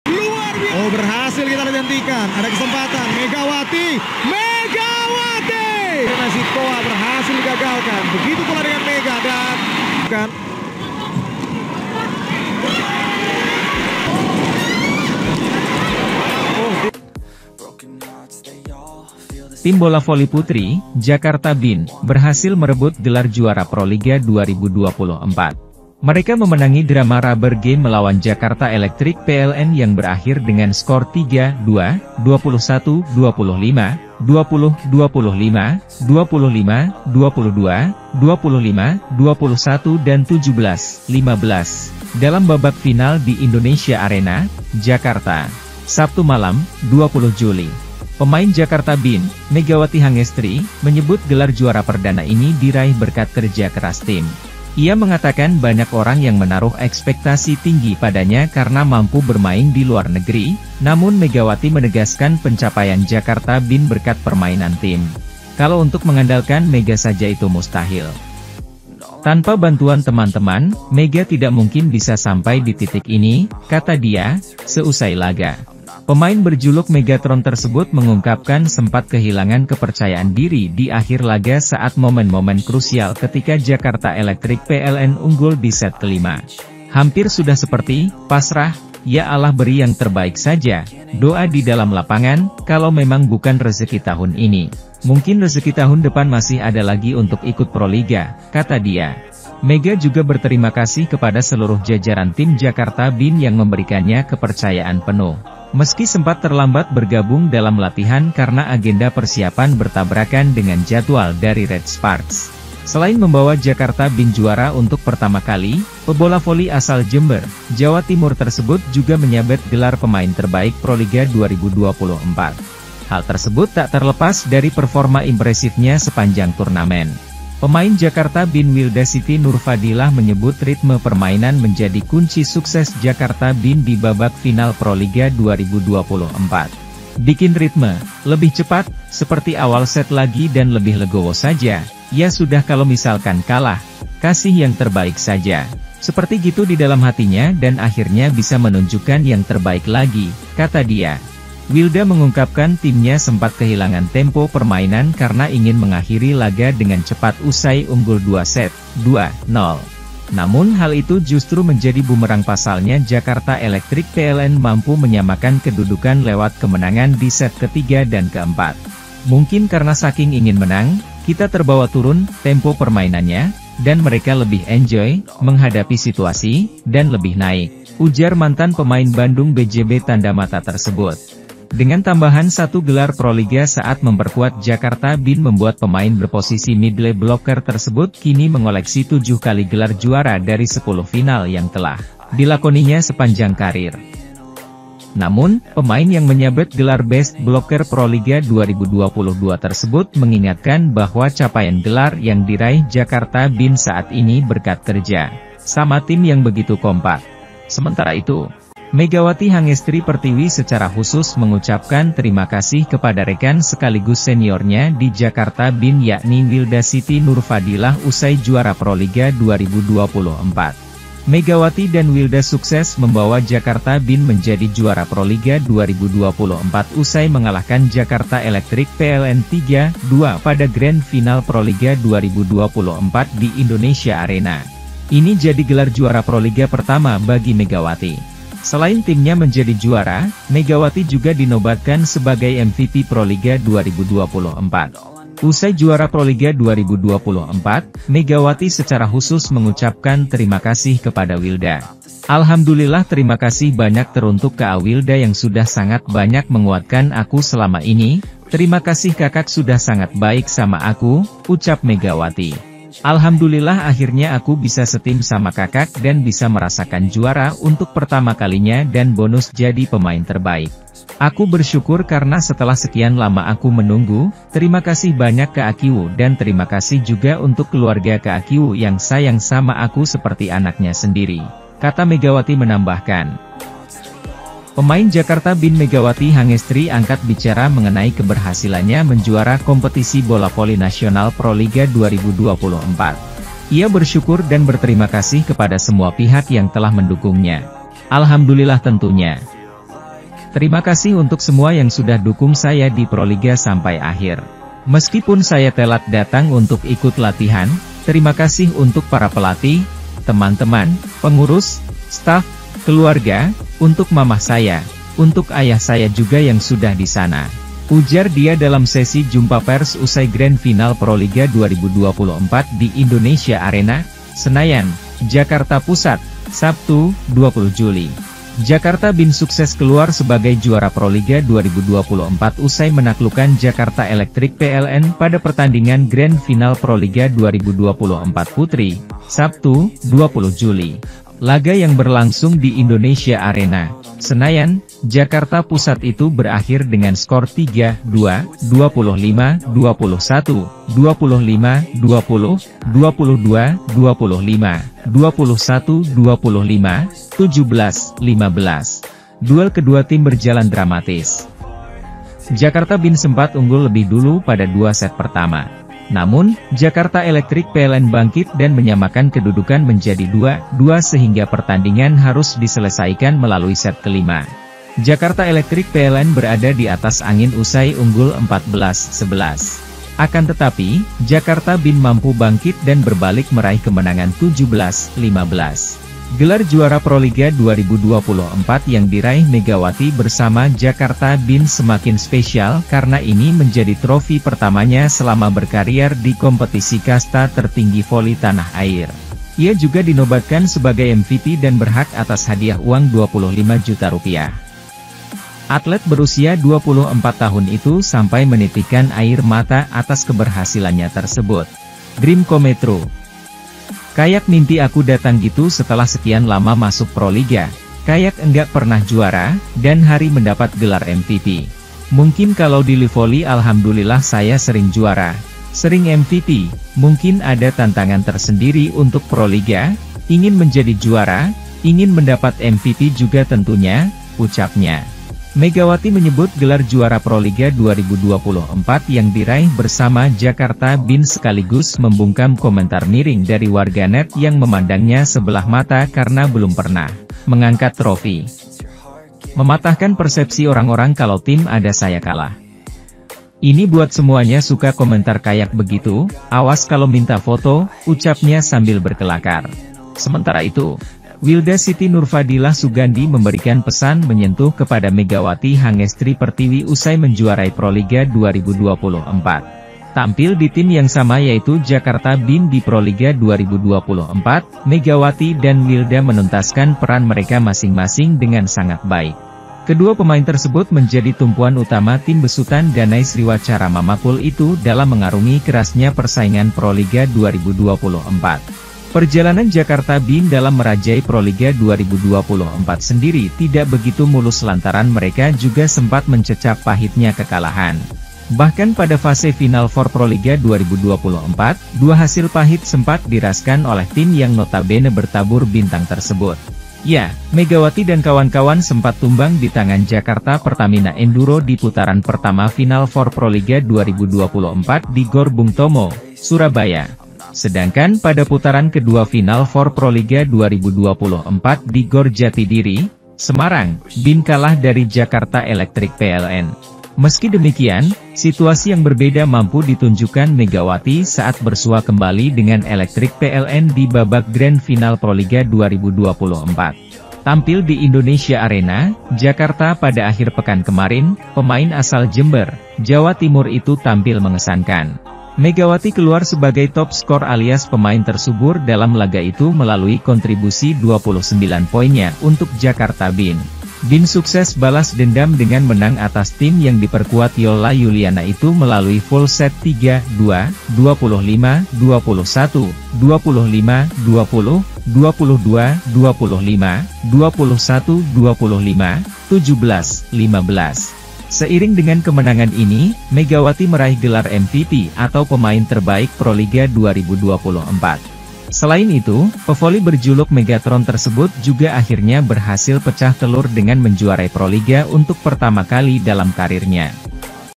VIRAL❗Curhat Tak Percaya Wilda Siti Nurfadilah Bintang JKT BIN, Melihat Heroiknya Mega jdi Pahlawan😱
TERIMAKASIH🤩